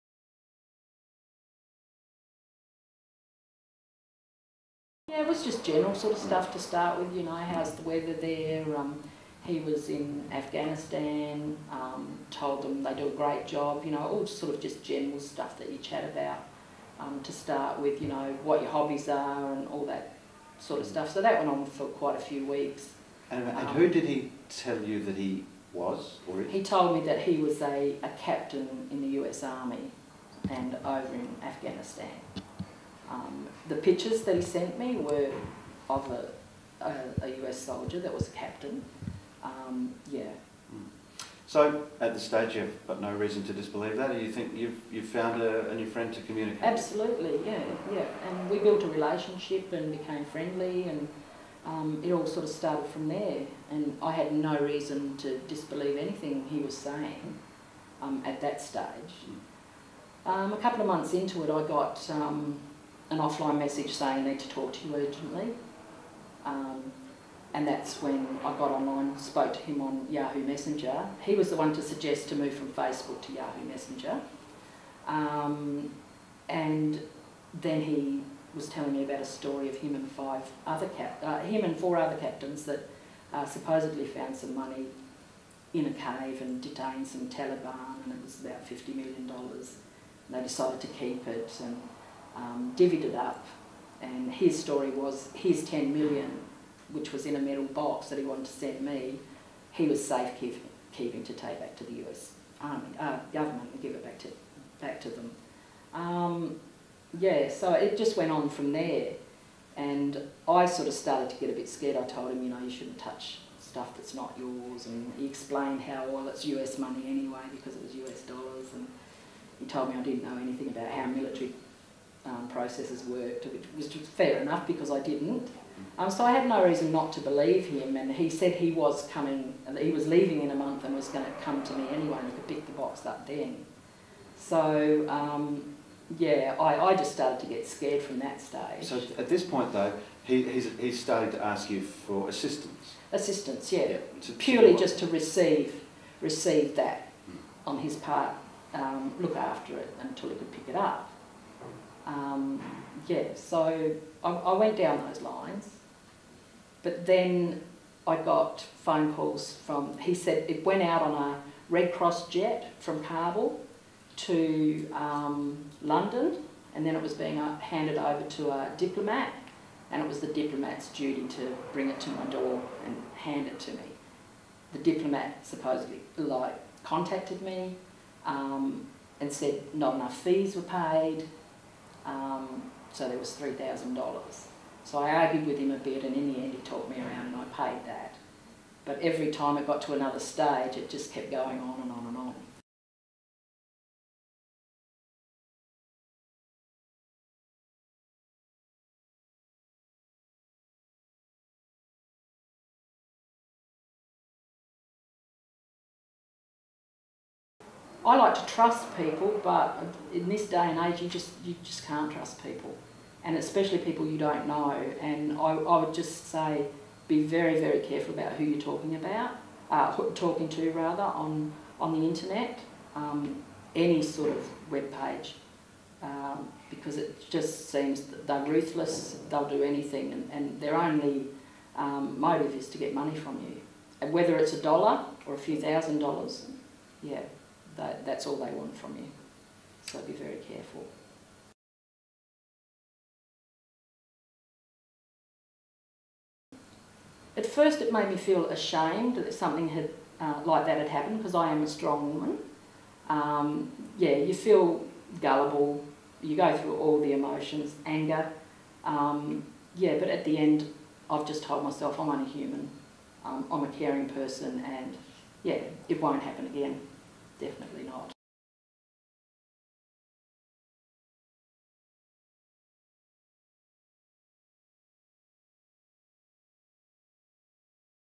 Don't get scammed like 'Karen'
South Australia Police will be sending letters to people who may have sent money overseas as a result of being targeted by online scammers in a bid to prevent them losing further funds.
So far this year South Australians have lost nearly a million dollars in 84 different scams which have been reported to police.
One victim, 'Karen', detailed to SA Police News how she met a man on Facebook who claimed to be a US soldier in Afghanistan, but who turned out to be a scammer.
He detailed a plan to remove cash from Kabul which began with a request that she simply store the money, but evolved into requests for money and over seven months he conned her out of more than $23,000 -- money she never expects to see again.
When she accused him of being a scammer he admitted he was Nigerian, not American, but said he was in love with her.
"I like to trust people but in this day and age you just can't trust people, especially people you don't know," she warns.